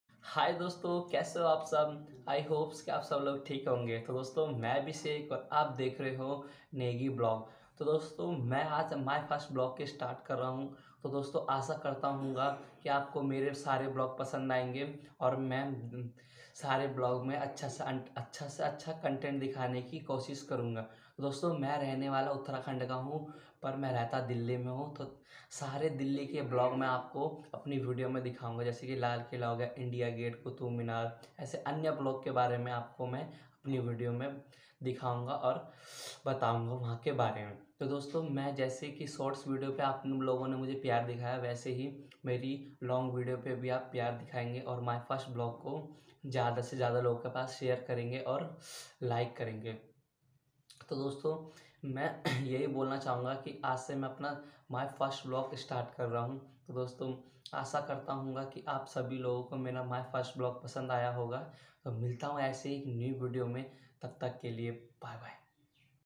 My first vlog 2023 | @negi vlog96
my description🤔🤔

    मेरा ये first व्लॉग बनाया है | ओर इस वीडियो मै
    अपने बारे मे बतया है | अगर मेरी वीडियो अच्छी
   लगी तो इस वीडियो को जादा जादा से share करे और like or Coment करे ?

keyword ❤❤
my Vlog
Vlog video
sorav joshi Vlog
sorav joshi Vlog video

thku for waching 🙏🙏❤

my instagarm I'd = abhishek negi 9667